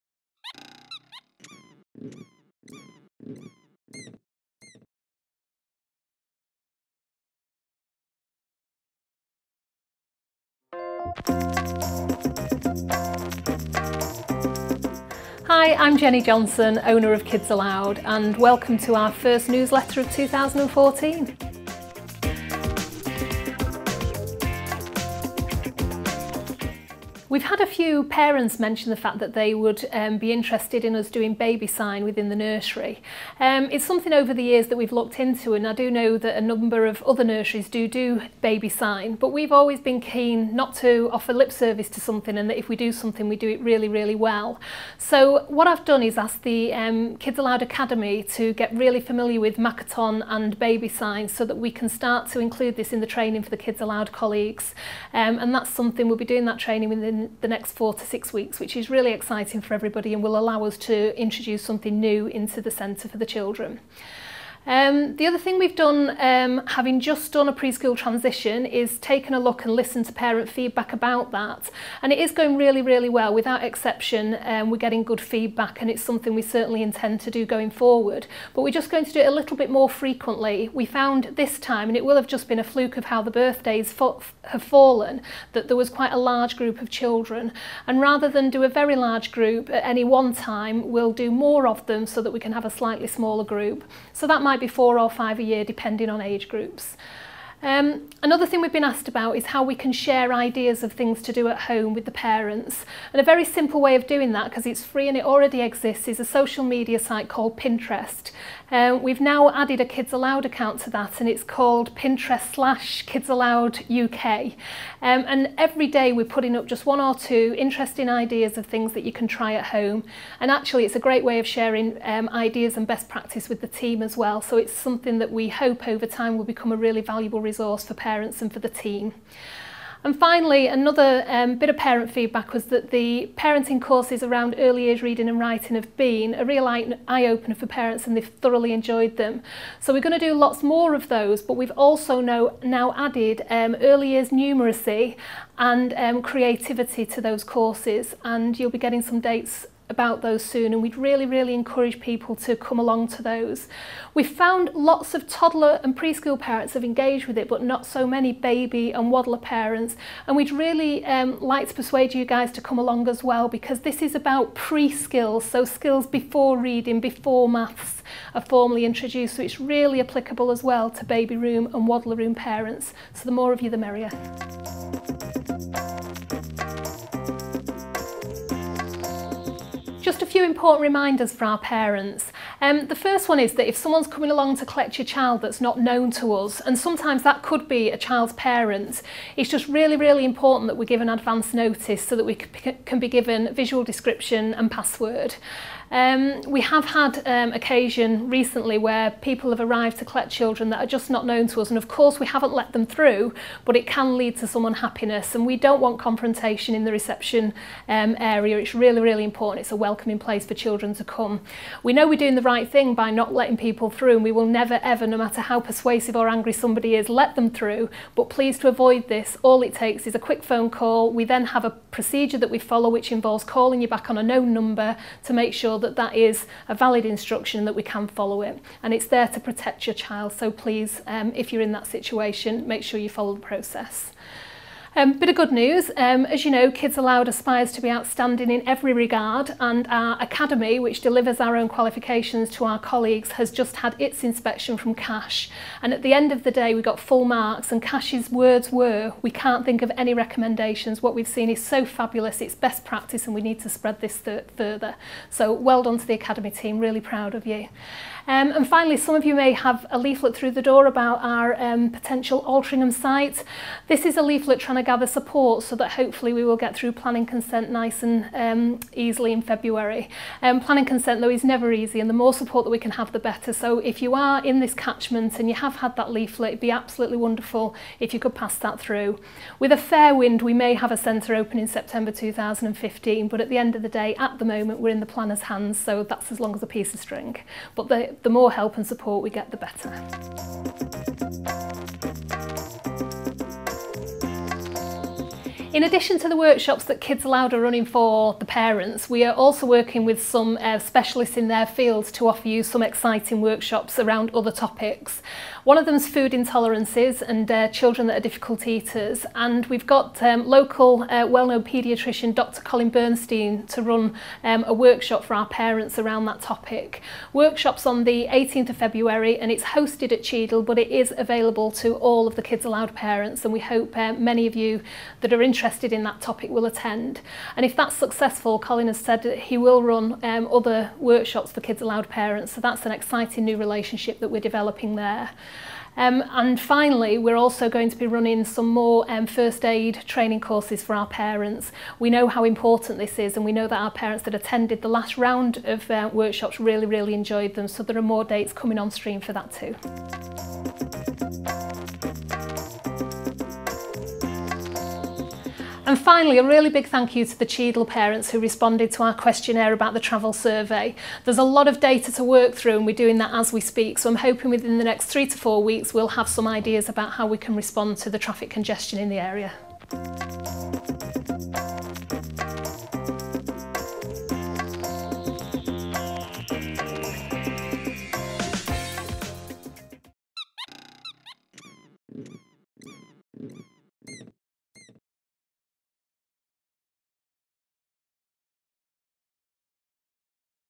Kids Allowed Parent Video Newsletter January 2014
Welcome to the Kids Allowed video newsletter, brought to you every month by our Founder and CEO Jennie Johnson.

Our January 2014 newsletter includes:

- Pre-school transitions update
- New parenting courses

- Academy inspection update
- New Altrincham site update

PART 3: Dates For Your Diary
- New parent workshops - see your own centre weekly newsletter for details

PART 4: And finally...
- Cheadle traffic congestion survey update